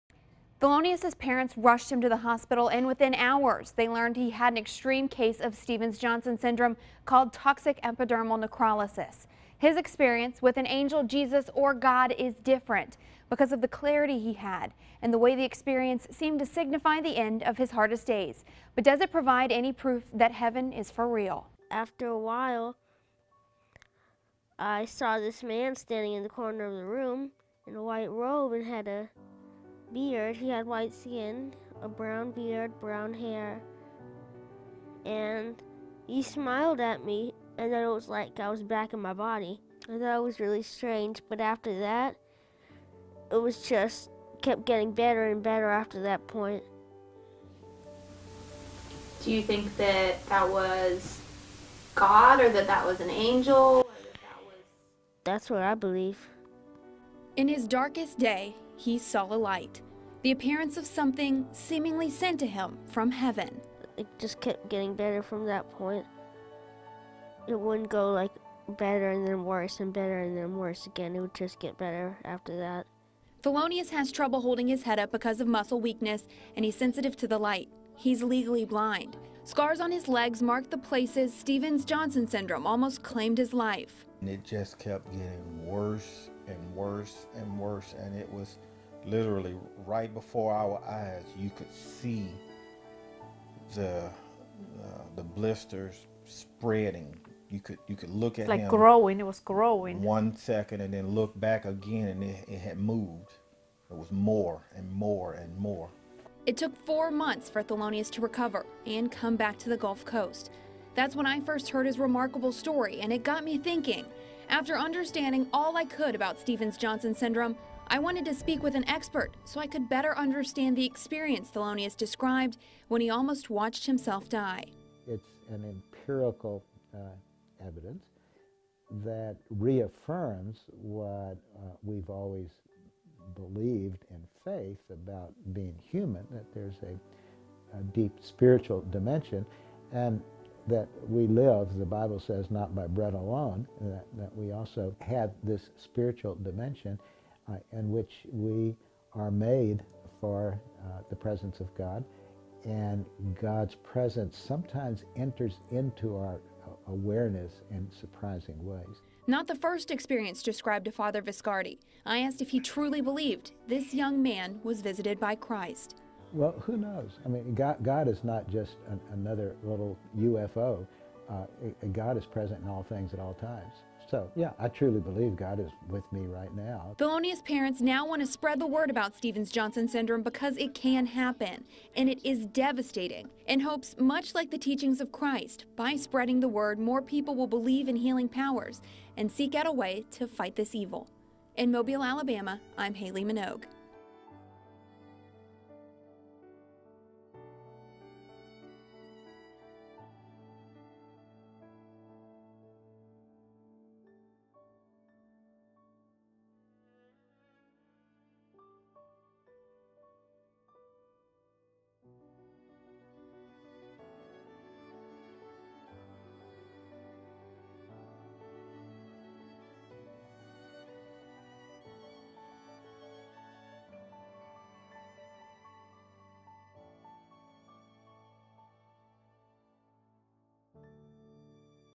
Is Heaven Real? Alabama boy hospitalized for months says he saw angel or God in his room
WKRG News 5 Special Report